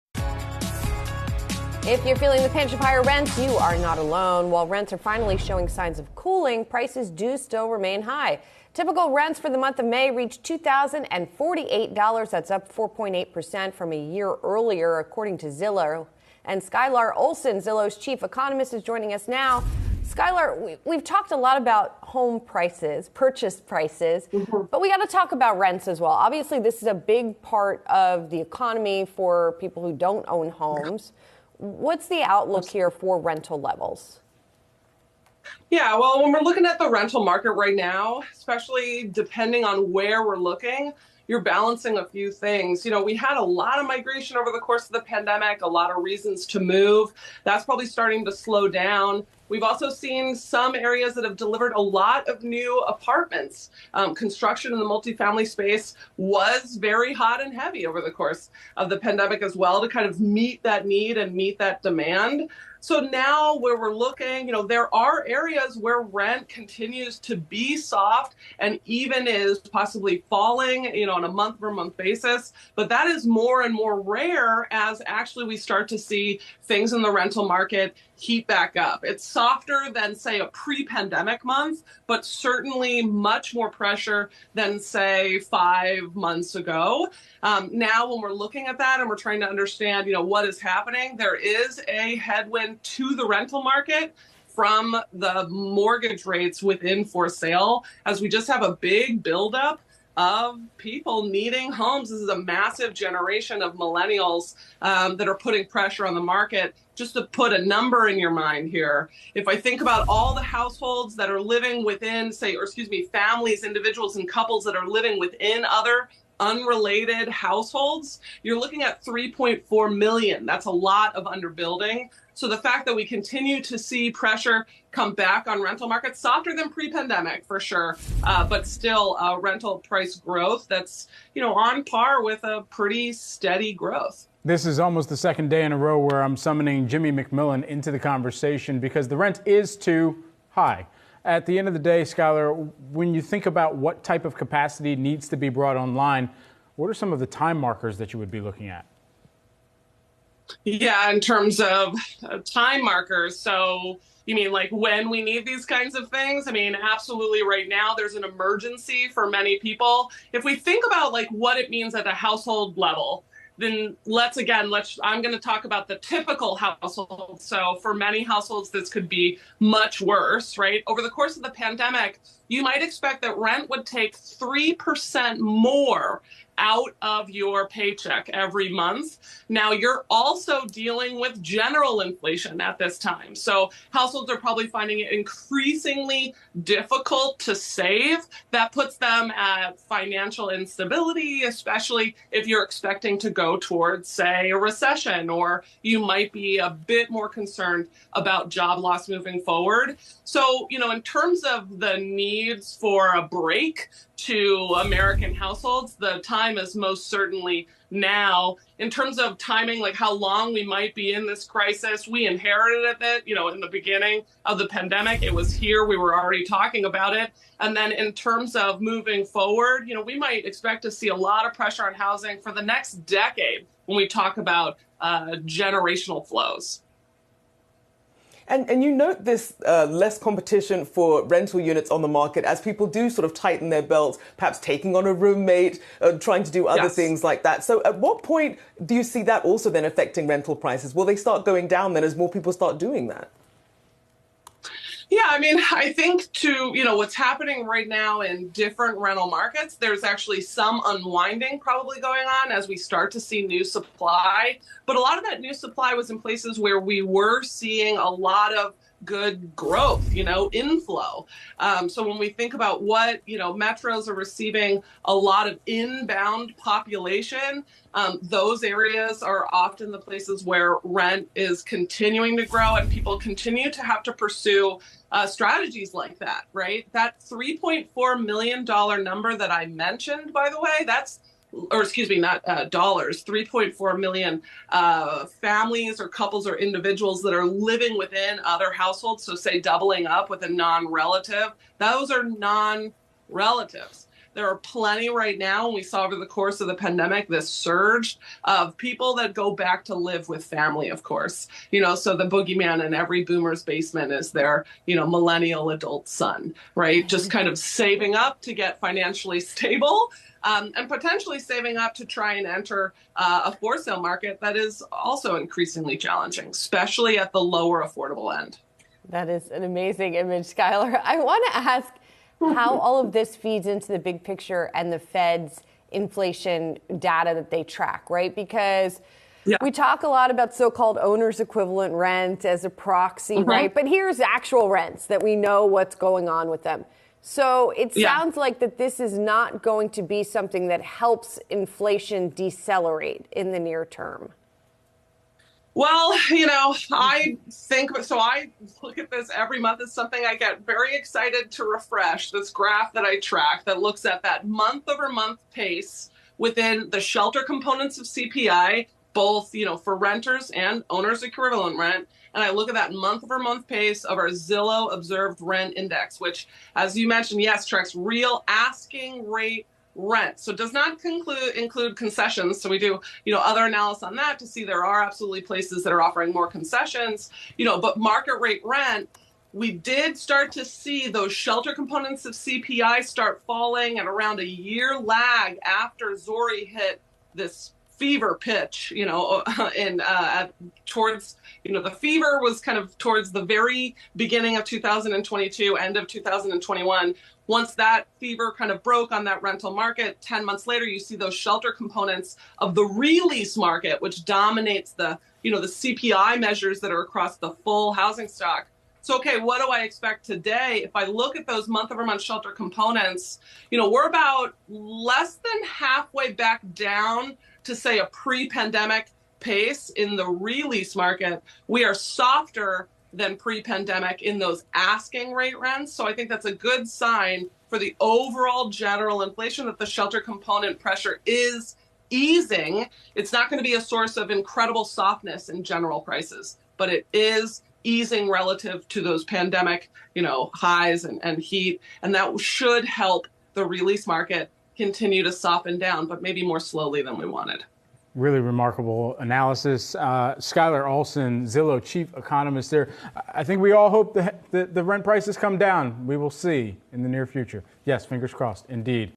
Rent inflation creating an 'emergency for many people': Economist
This segment originally aired on June 15, 2023.
Zillow Chief Economist Skylar Olsen spoke with Yahoo Finance anchor Julie Hyman about how inflation, mortgage rates are impacting the ability to afford housing for some.